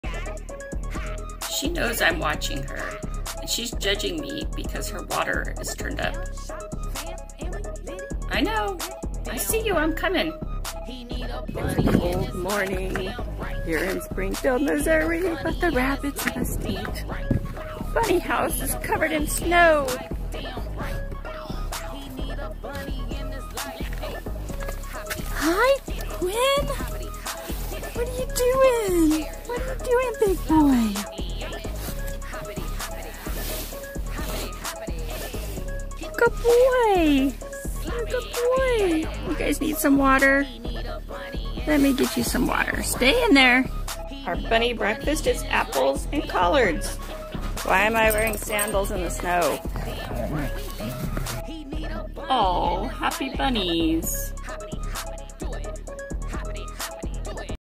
My Rabbits Are Judging Me!!
I woke up, checked the Bunny Cam and found Calico giving me the side eye about the current water situation.